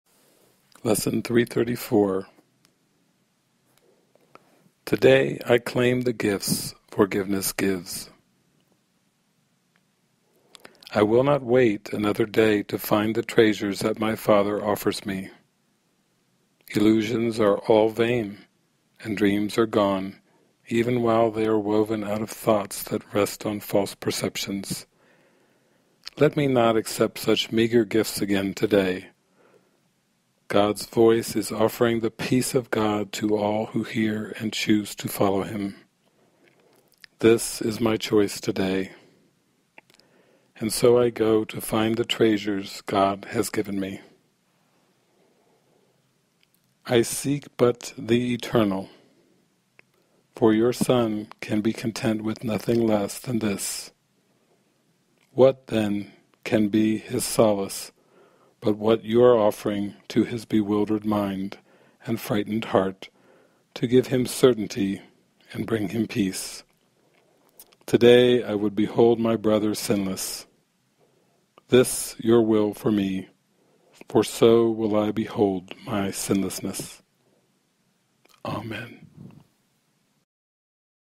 ACIM Lesson - 334 "Today I claim the gifts forgiveness gives" by David Hoffmeister
Practice and live the mystical teachings of Jesus assisted by online courses, online retreats, and an online Tribe of Christ community: David Hoffmeister, who speaks from the Awakened Mind, continues a journey through the spiritual classic A Course In Miracles by reading from The Workbook, Lesson 334 "Today I claim the gifts forgiveness gives".